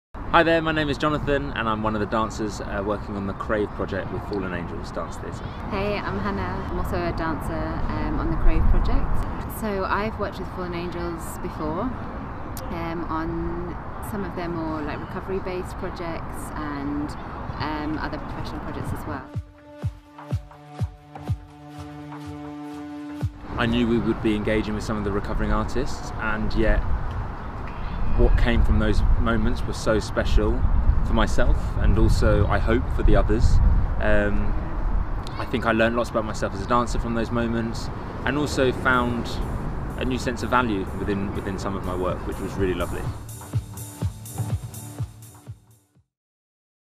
Behind the Scenes of Transfiguration: I Fall - Jon Savage and Hannah Rudd
Transfiguration is our brand-new trio of 10-minute dance films, focusing on a series of defining moments in the journey from addiction to recovery.

Transfiguration: I Fall will be available to watch for free via StoryhouseLive from Thursday 20 January 2022.

I Fall explores the carnage and chaos of addiction. Focusing on the twisted love story between two addicts – how they can’t live with or without each other – and their unbearable compulsion to use.

But through the despair, we see that recovery can be possible.

Sign up for our e-newsletter to be the first to watch